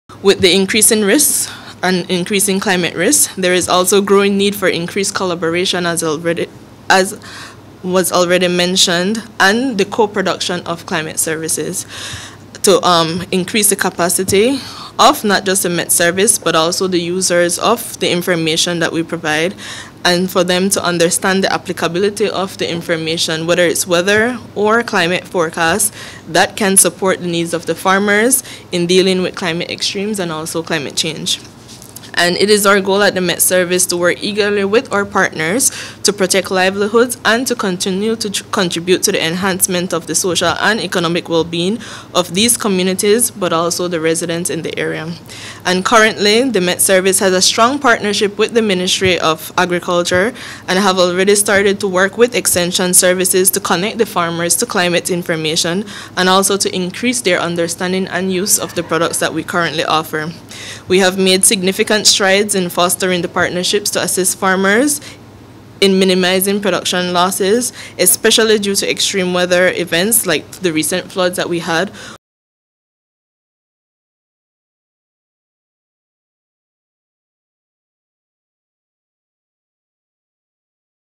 Ministry of Economic Development inks MOU with the Met Service | PT 2
The CIS will, therefore, provide farmers with timely and accurate climate information, allowing them to plan their activities accordingly and minimize climate-related losses to their crops. Senior Climatologist, Shanea Young added that farmers will access critical information in real-time to help them make better decisions.